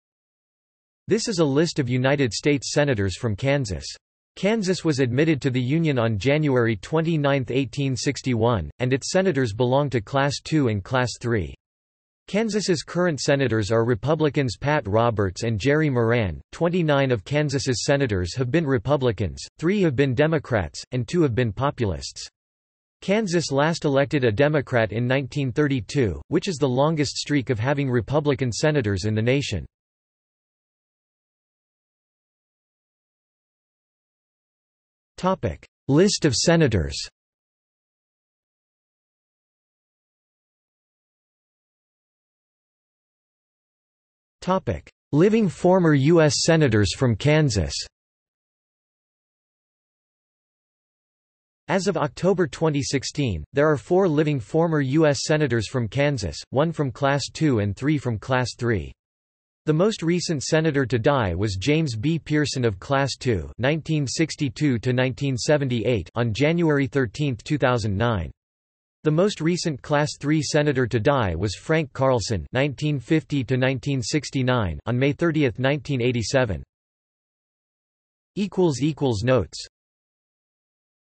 List of United States Senators from Kansas | Wikipedia audio article
This is an audio version of the Wikipedia Article:
List of United States Senators from Kansas

00:00:31 1 List of Senators
00:00:40 2 Living former U.S. Senators from Kansas

- Socrates

SUMMARY
This is a list of United States Senators from Kansas. Kansas was admitted to the Union on January 29, 1861, and its senators belong to Class 2 and Class 3. Kansas's current senators are Republicans Pat Roberts and Jerry Moran. 29 of Kansas's senators have been Republicans, 3 have been Democrats, and 2 have been Populists. Kansas last elected a Democrat in 1932, which is the longest streak of having Republican senators in the nation.